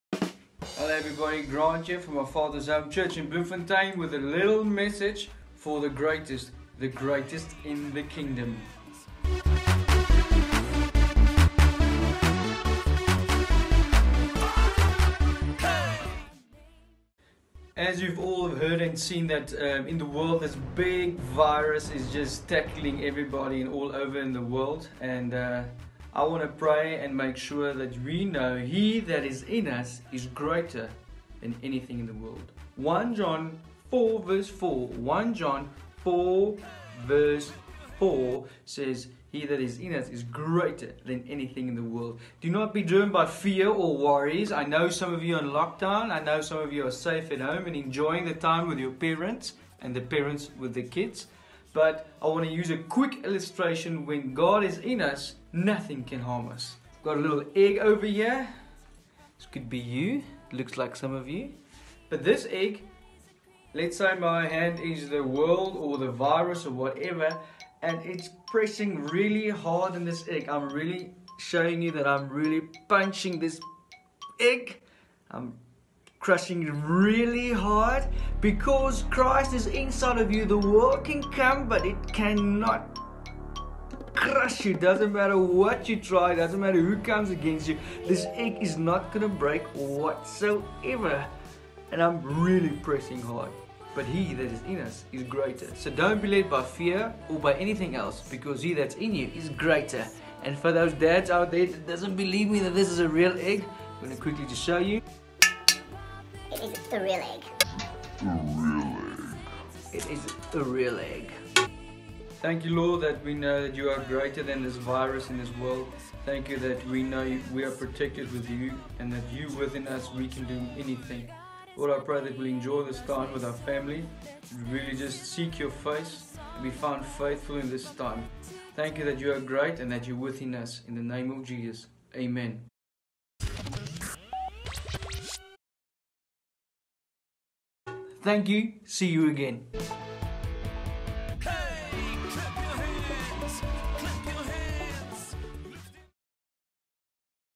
Children's Church - Egg Trick & Message by Grand Cloete (5 April 2020)
We invite the children to watch an amazing egg trick and see what happens when the world tries to crush us. Should we be afraid of this 'big virus'? Find out what 1 John 4:4 says.